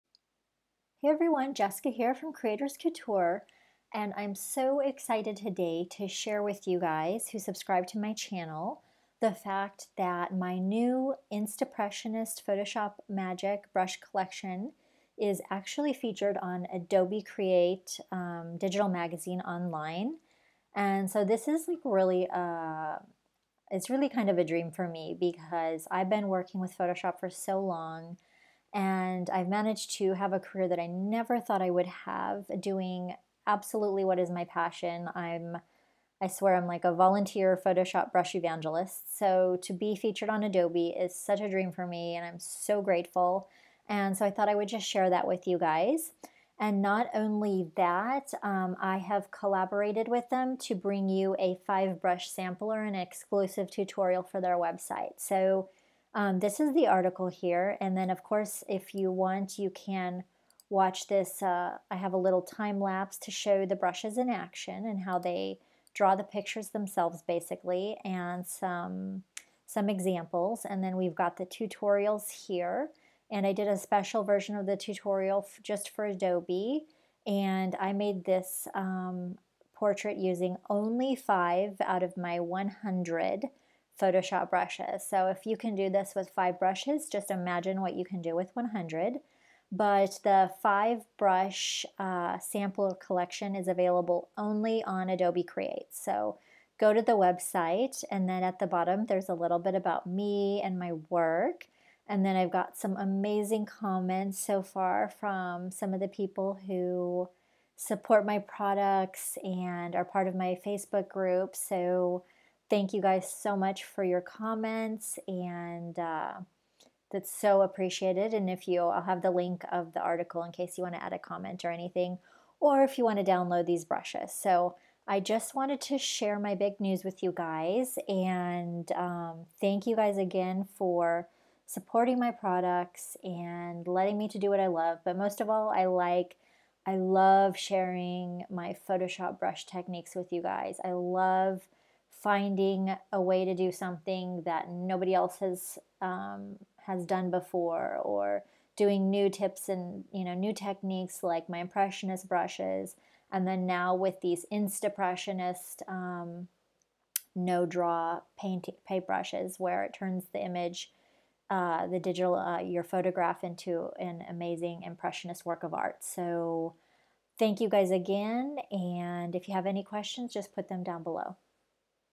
Adobe Create + Creators Couture | Download 5 FREE Instapressionist Photoshop Brushes
I'm so proud to share with you this special collaboration with Adobe Create! It's a dream to be featured on the Adobe blog and share this game-changing Photoshop brush technique with everyone!

Introducing "Instapressionist” Photoshop Brush Magic: A new breakthrough Photoshop brush collection by Creators Couture that allows Photoshop users of any skill level to turn a photographic image into creative and unique digital artwork. You simply grab a brush, start painting, and watch the magic unfold.